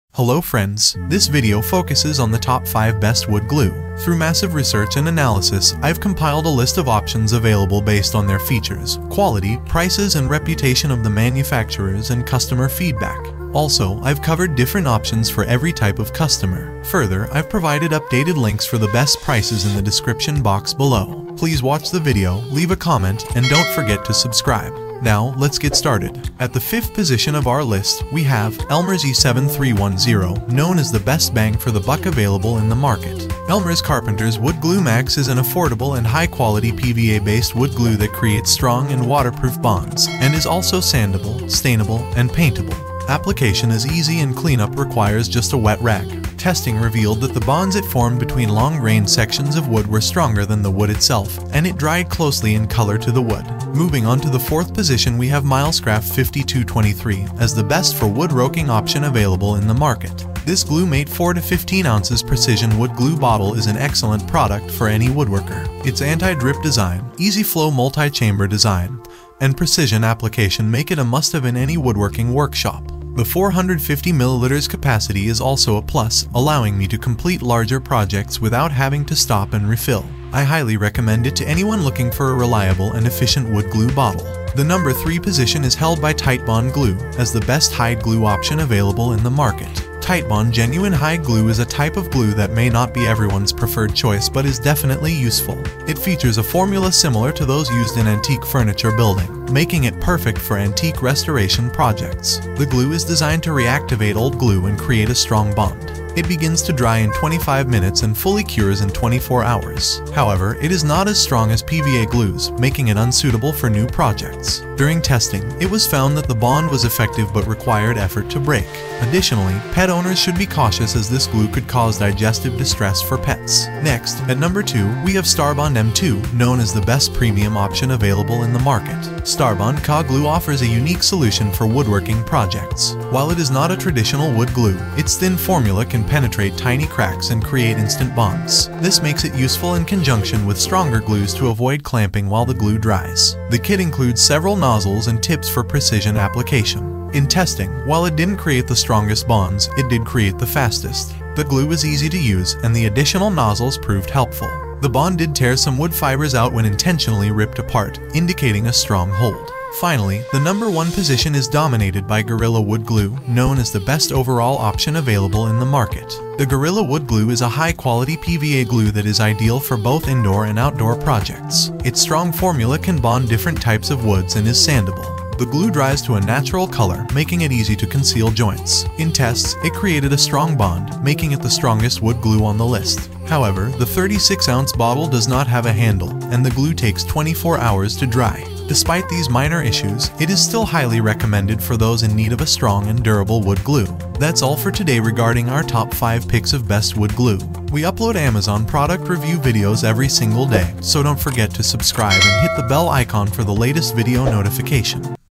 Top 5 Best Wood Glue of 2023
➜ Links to the Best Wood Glue of 2023 we reviewed in this video:

►Global Links◄
➜ 5. Elmer's E7310
➜ 4. Milescraft 5223
➜ 3. Titebond Glue
➜ 2. STARBOND EM-02
➜ 1. Gorilla Wood Glue
Wood glue is an essential tool for any woodworking project, whether you're a professional carpenter or a DIY enthusiast. With so many options available on the market, it can be tough to choose the right one for your needs. That's where we come in! We've tested and compared the most popular wood glues on the market to bring you our top 5 picks. In this video, we'll be discussing the pros and cons of each glue and what makes them stand out. We'll also be covering important factors to consider when selecting a wood glue, such as drying time, strength, and water resistance. Whether you're looking for a budget-friendly option or a high-performance glue, we've got you covered. Our top 5 picks offer a range of options to suit every need and budget. So, if you're in the market for a new wood glue, be sure to check out our video for expert recommendations and helpful insights.